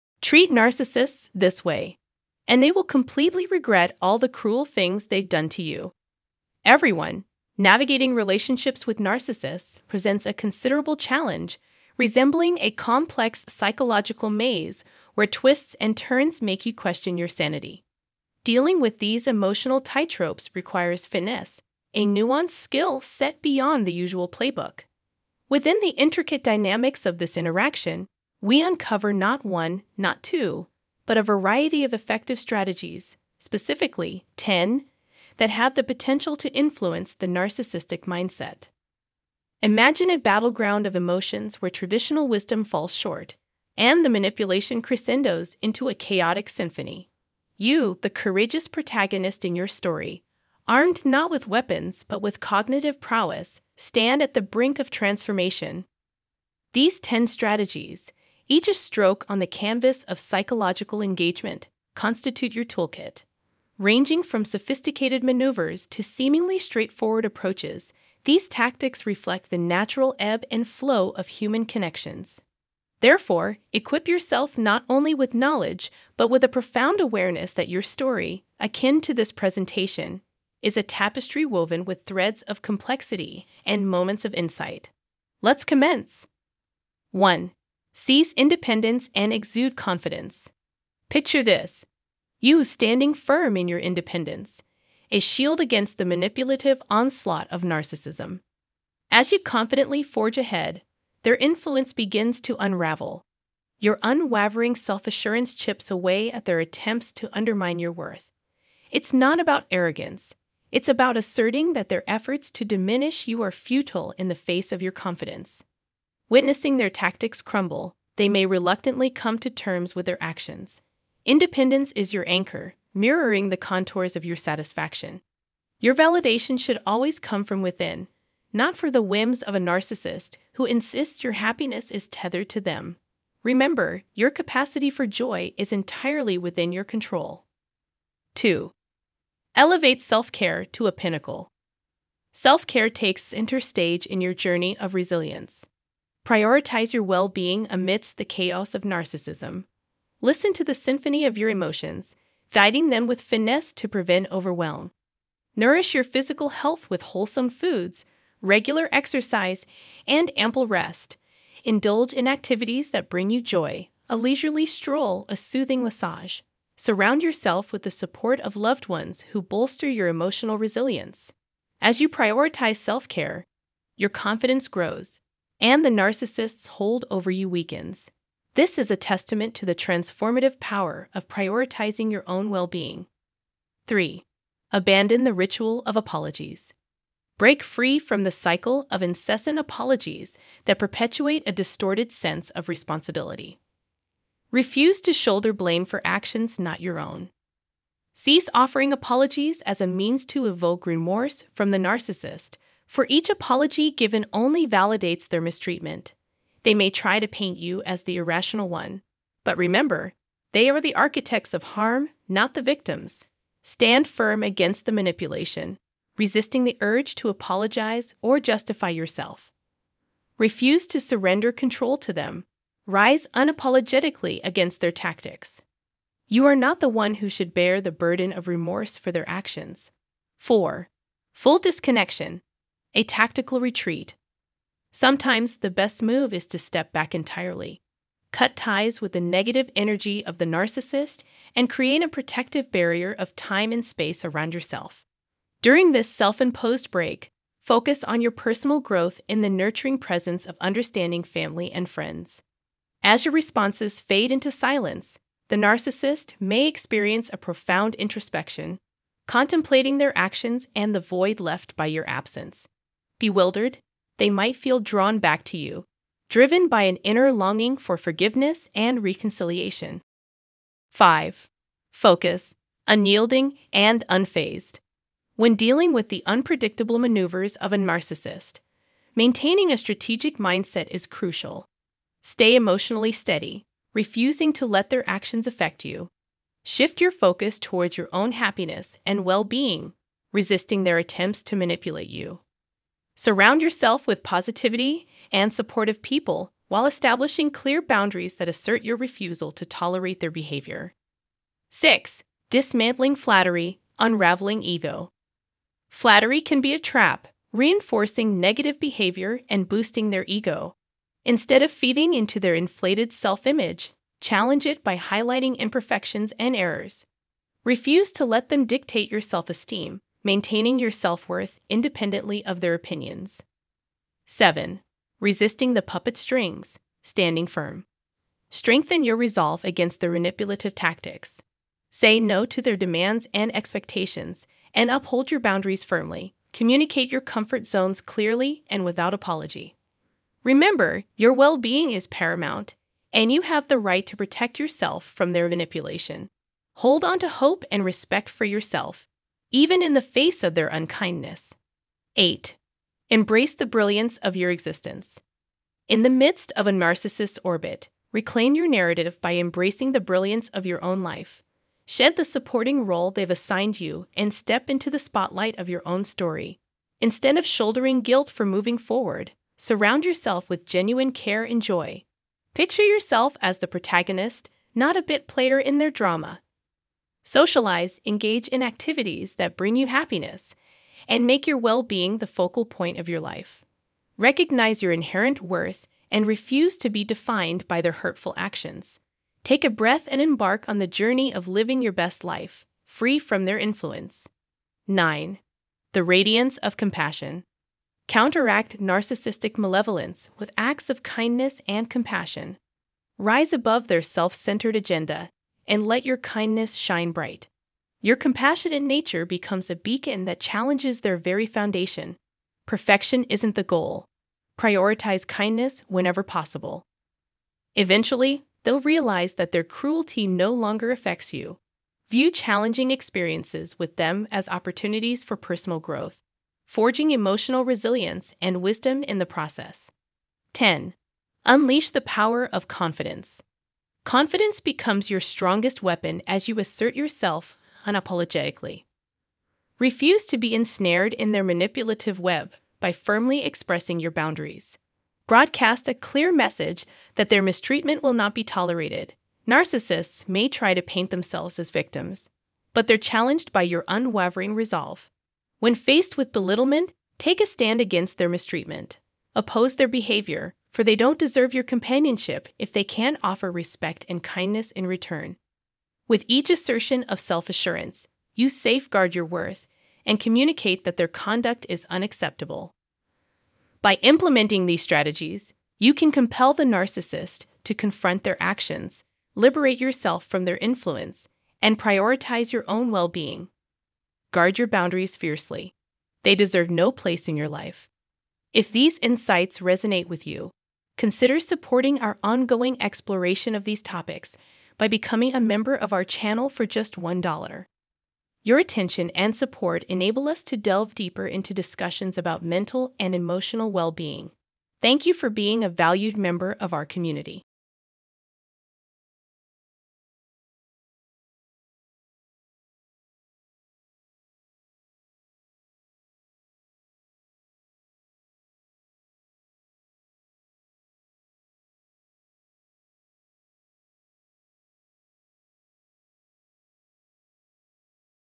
Treat Narcissists This Way, and They Will Completely Regret All the Cruel Things They've Done to You
Treat Narcissists This Way, and They Will Completely Regret All the Cruel Things They've Done to You | NPD | Narcissism Backfires